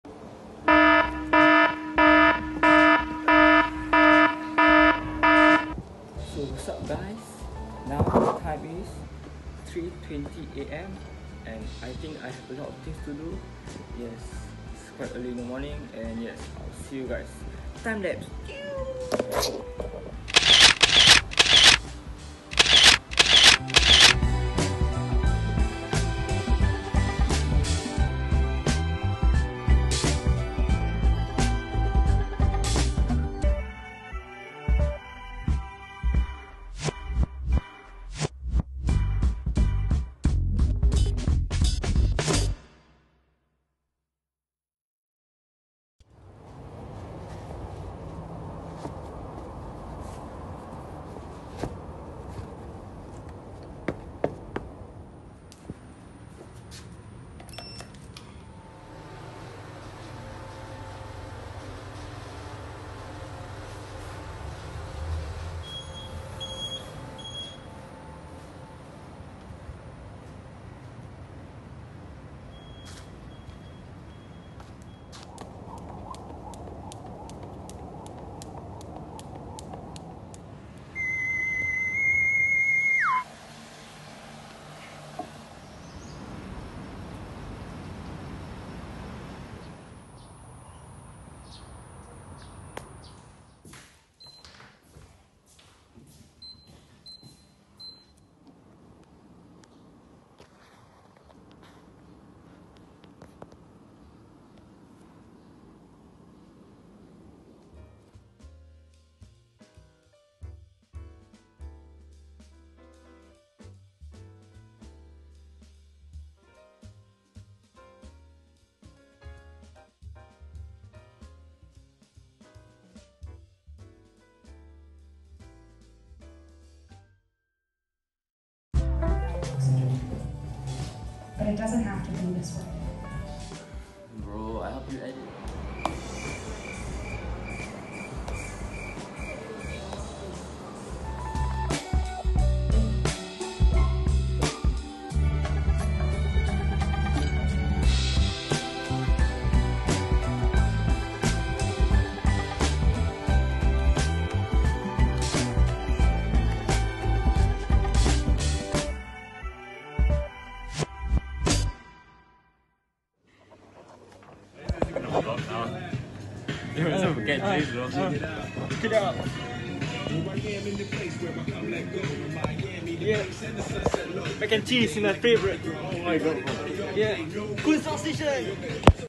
A day in a life of a Secondary 5 Student
This vlog is inspired by @DanialTamago ,
This is my very first vlog so don’t judge HAHAHAHAHA
testing jerr